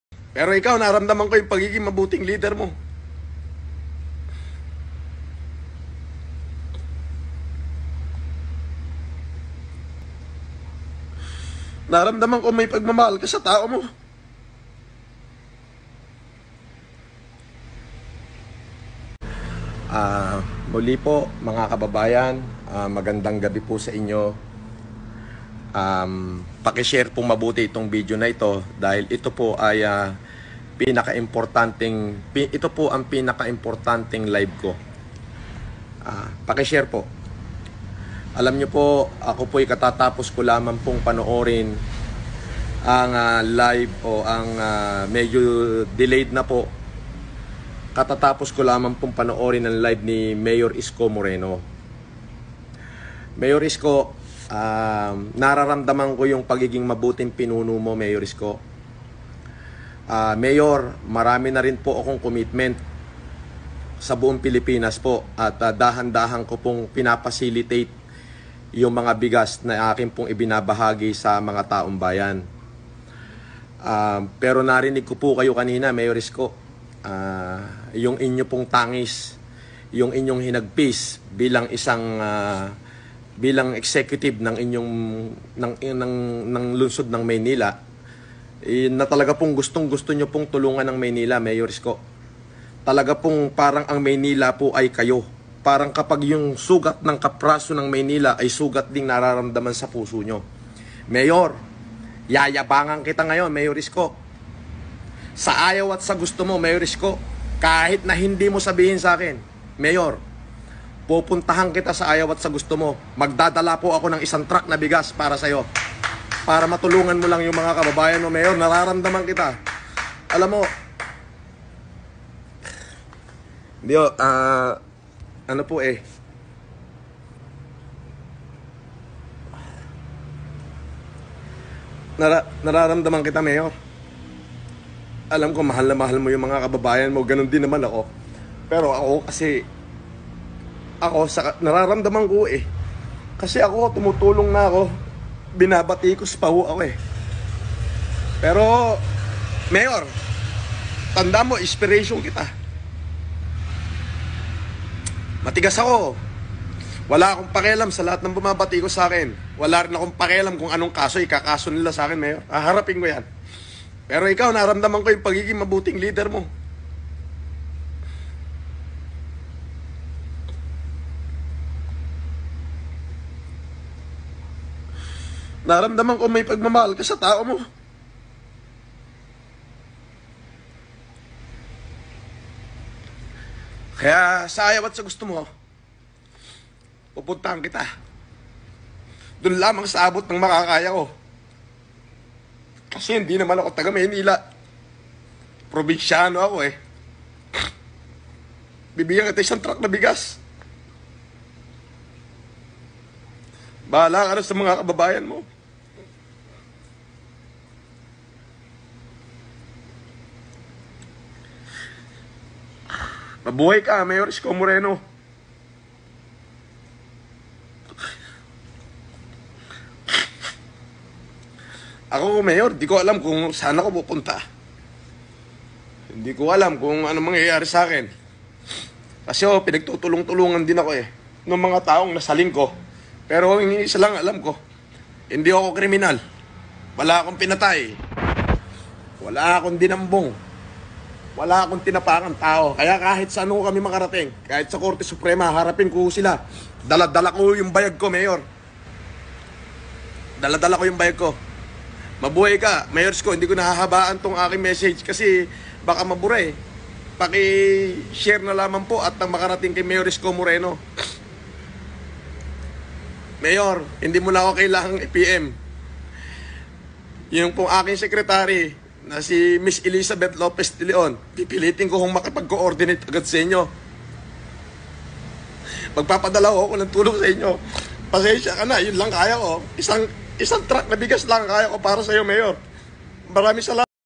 Francis leo Marcos ng donate ng 1 truck ng bigas para sa Maynila!!! Yorme Nagpasalamat!!
francisleomarcos iskomoreno mayorisko francisleomarcosvlog mayaman challenge